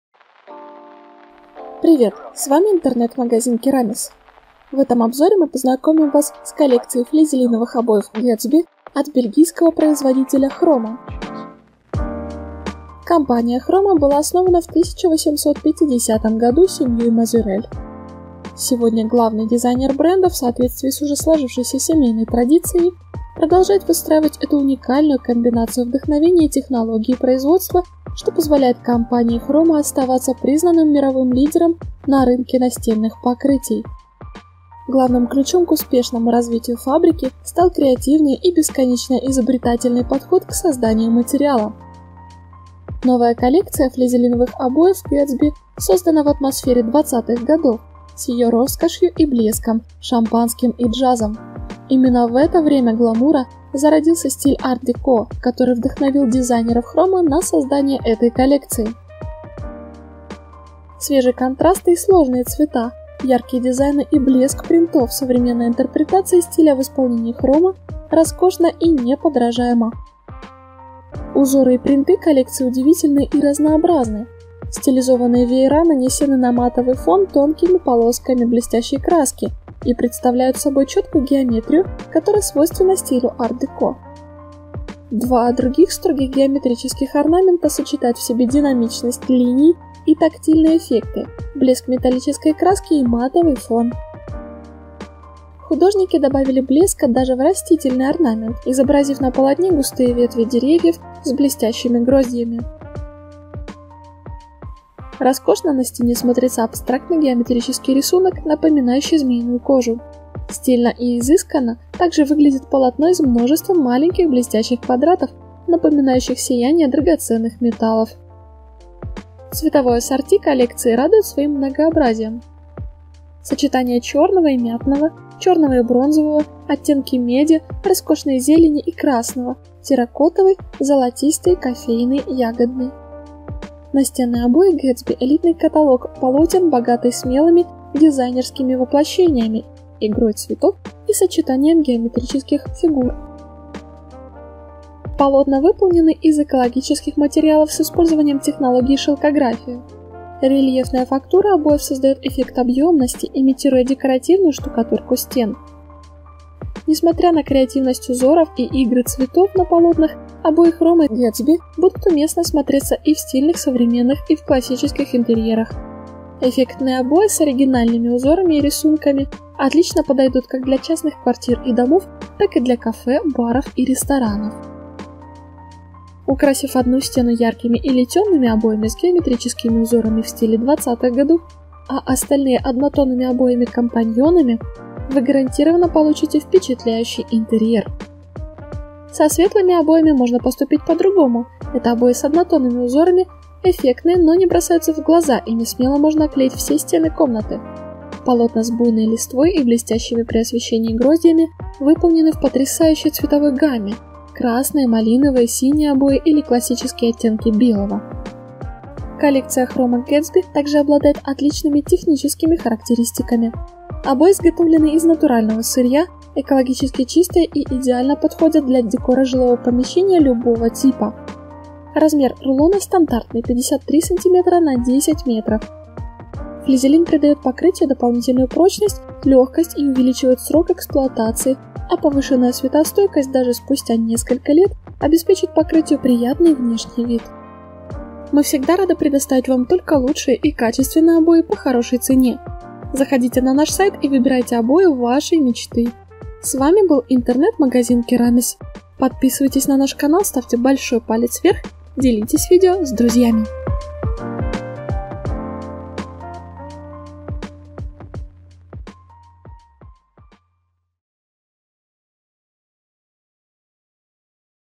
Впечатляющий интерьер с гламурными обоями Khroma Gatsby
Настенные обои Gatsby – элитный каталог полотен богатый смелыми дизайнерскими воплощениями, игрой цветов и сочетанием геометрических фактур. Полотна выполнены из экологических материалов с использованием технологии шелкография. Рельефная фактура обоев создает эффект объемности, имитируя декоративную штукатурку стен. Несмотря на креативность узоров и игры цветов на полотнах, обои Khroma из каталога Gatsby, будут уместно смотреться и в стильных современных, и в классических интерьерах.

Краткие характеристики:
Страна
Производитель
Коллекция: Gatsby
Ширина рулона: 53 см
Длина рулона: 10,05 м.п
Материал обоев